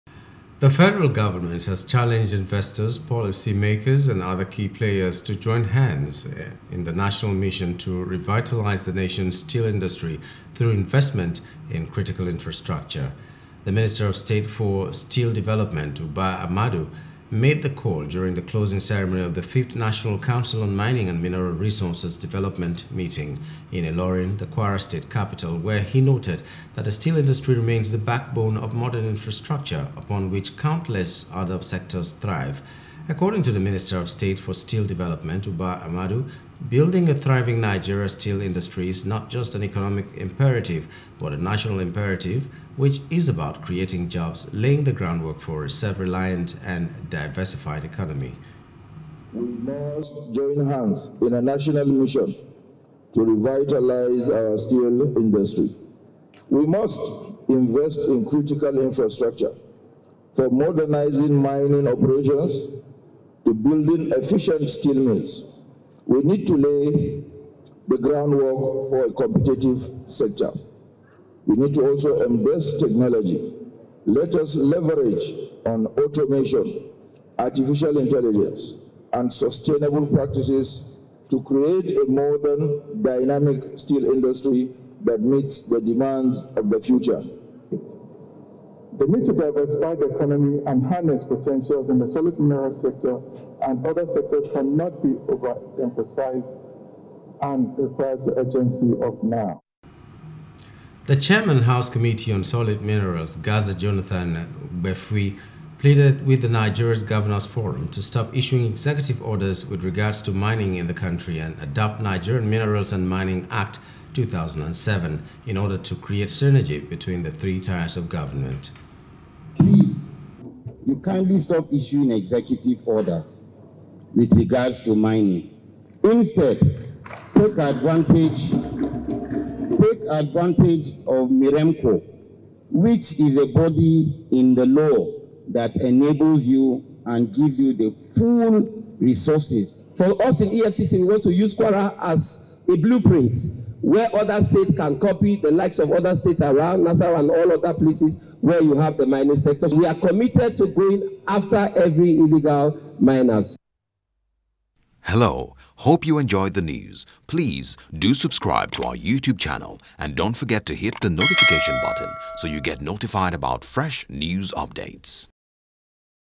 Steel Sector: F.G Tasks Key Players To Invest In Tech & Infrastructure
The Federal government has challenged investors, policy makers and other key players to join hands in the National mission to revitalize the Nation’s steel industry through investment in critical infrastructure.